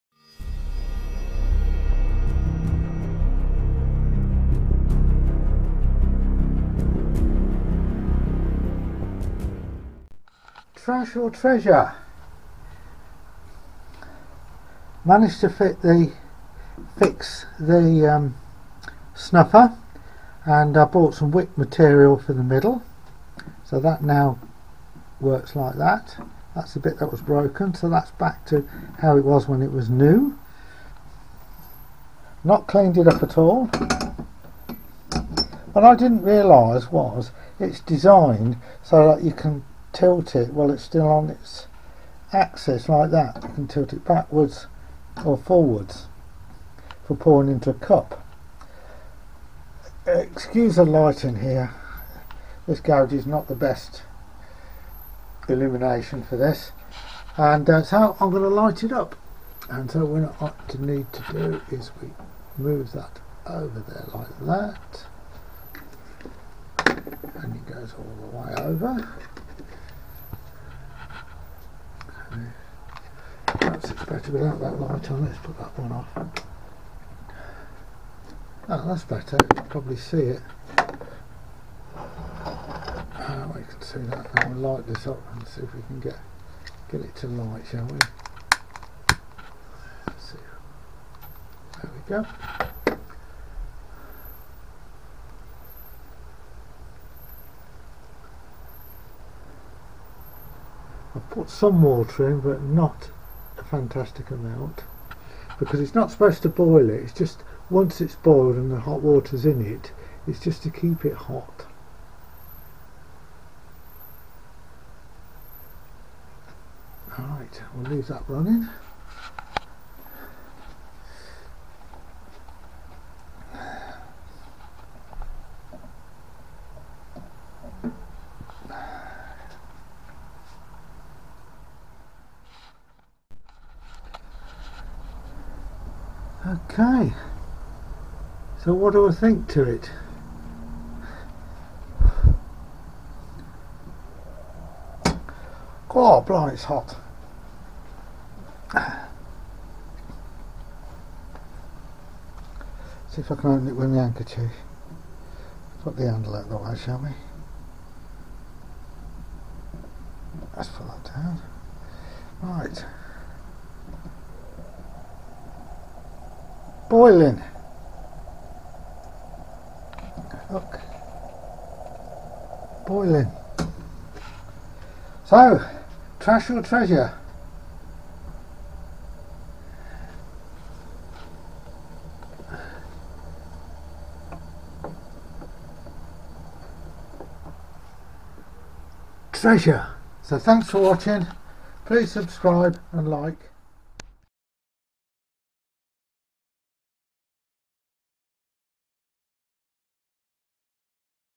Spirit Kettle. Trash or Treasure. Charity Shop Find. charity shop
Found in a Charity Shop. Turns out to be Treasure not Trash as now with a bit of DIY fixing it works as good as new.